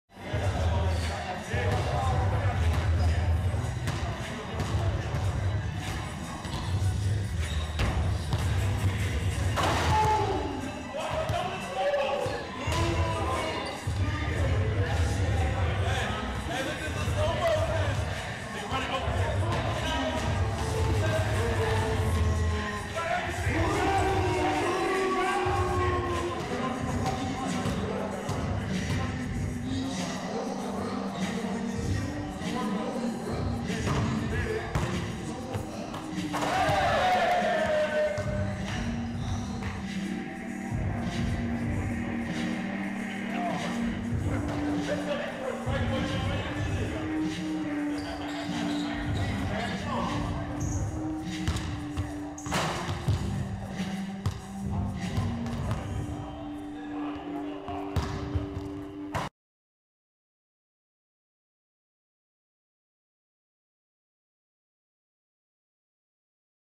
LeBron Puts on a Dunking Clinic in Practice
LeBron James exhibits his high-flying prowess during a team practice. for more highlights.